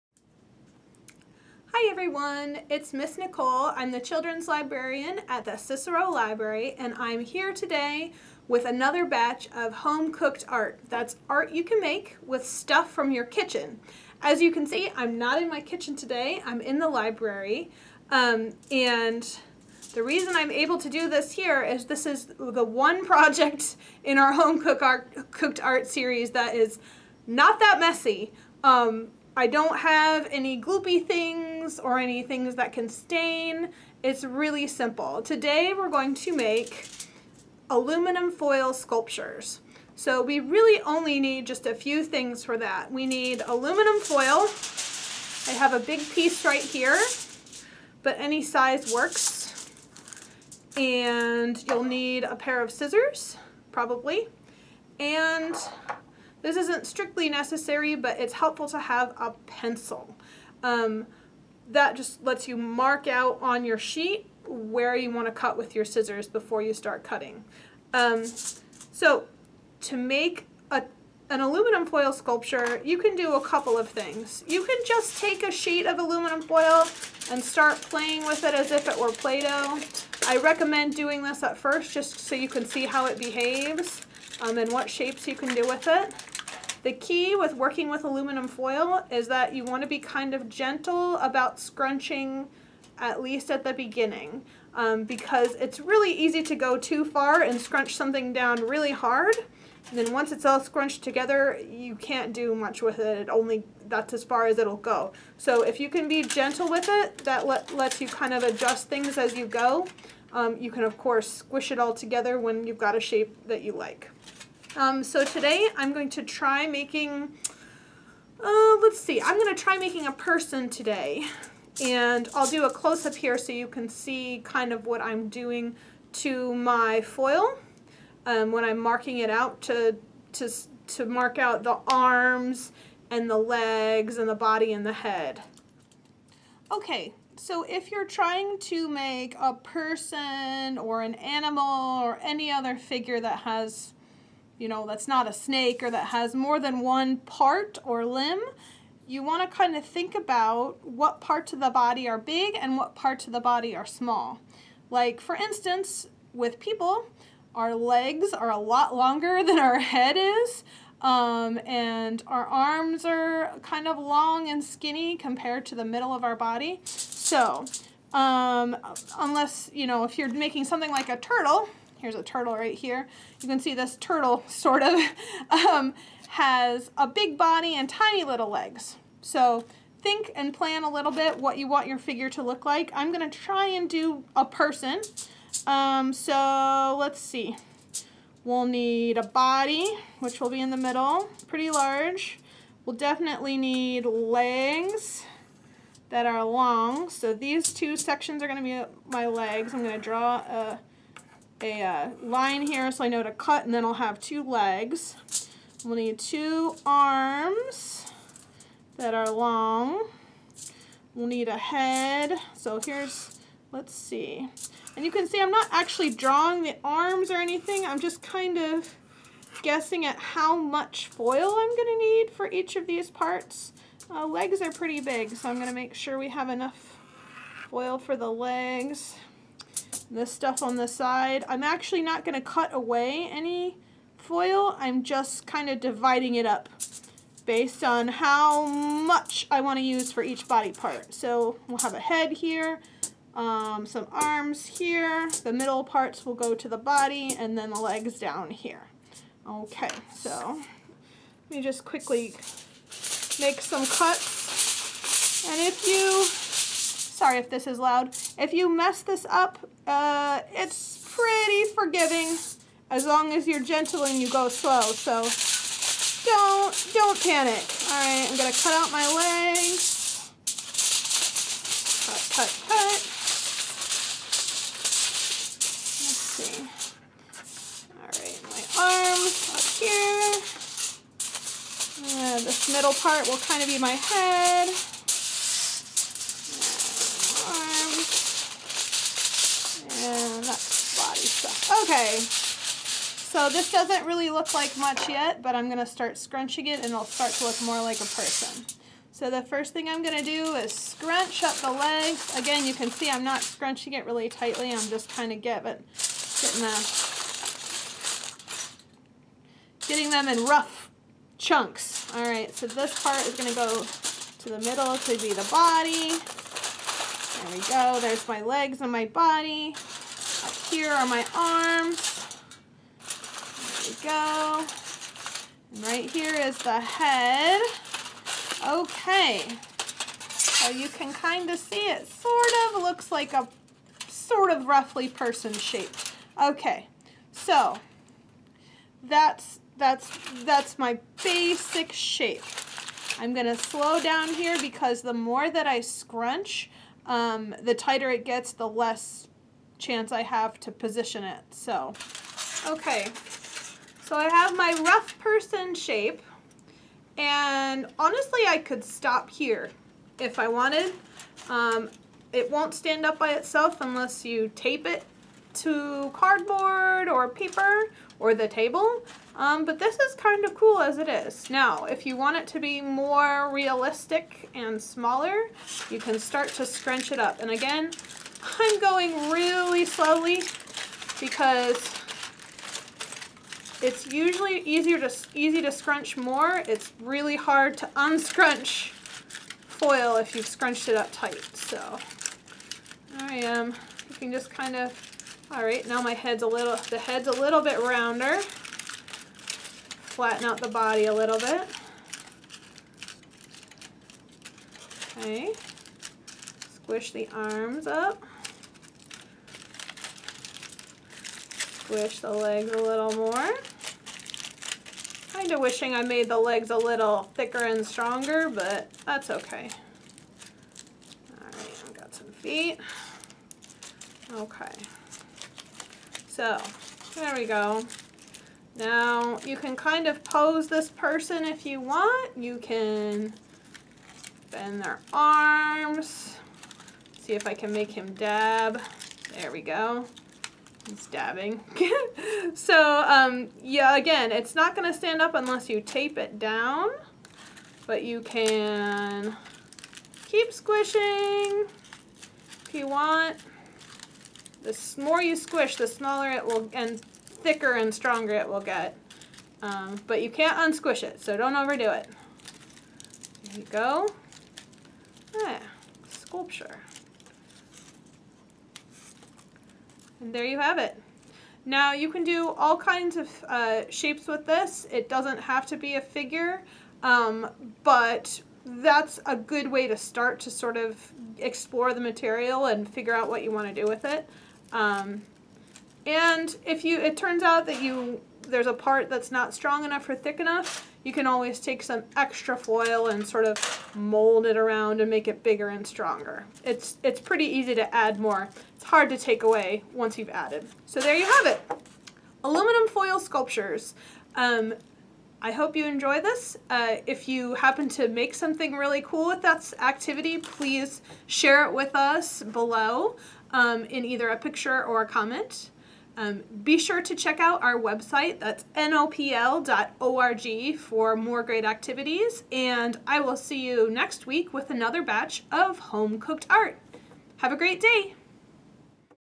Homecooked Art: Aluminum Foil Sculptures
Make art with materials you probably have in your own kitchen. For ages preschool through adult.